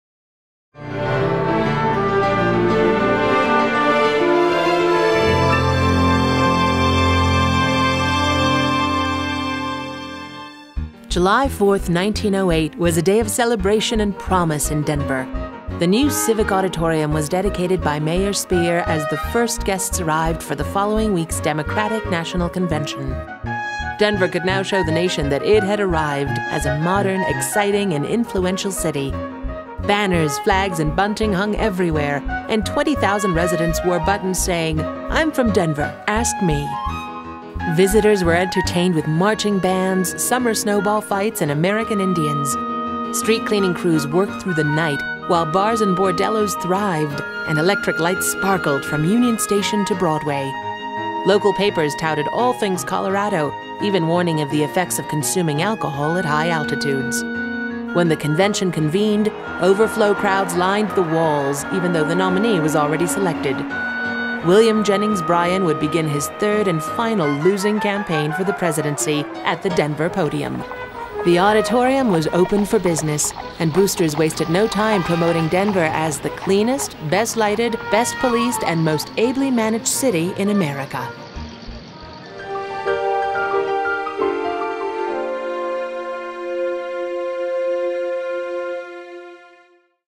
History Minute - 1908 DNC part 02
A short program on the unique history of Denver